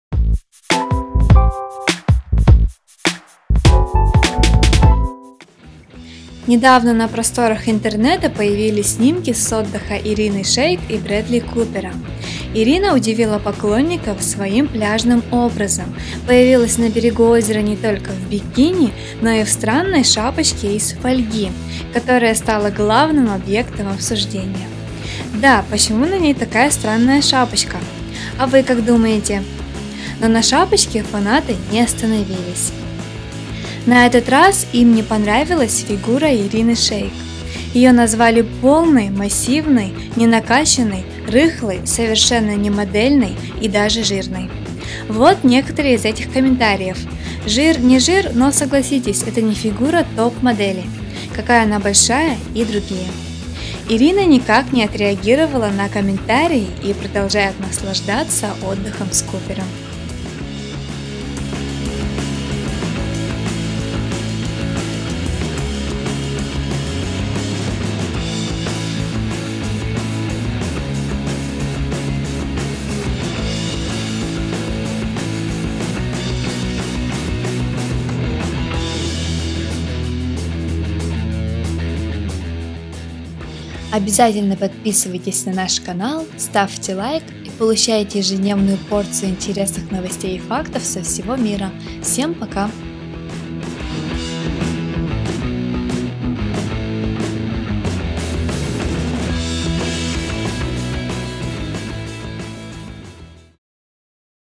ИРИНА ШЕЙК ШОКИРОВАЛА ПОКЛОННИКОВ СВОЕЙ ФИГУРОЙ / инстаграм, располнела, фото, бредли купер, роналду
Недавно на просторах Интернета появились снимки с отдыха Ирины Шейк и  Брэдли Купера (Bradley Cooper). Ирина удивила поклонников своим пляжным образом — появилась на берегу озера не только в бикини, но и в странной шапочке из фольги, которая стала главным объектом обсуждения. Да, почему на ней такая странная шапочка? А вы как думаете? Но на шапочке фанаты не остановились.

На этот раз им не понравилась фигура Ирины Шейк. Её назвали «полной», «массивной», не накачанной», «рыхлой», «совершенно не модельной» и даже «жирной».

Вот некоторые из этих комментариев: «Жир, не жир, но согласитесь, это не фигура топ-модели», «Какая она большая» и другие.

Ирина никак не отреагировала на комментарии и продолжает наслаждаться отдыхом с Купером.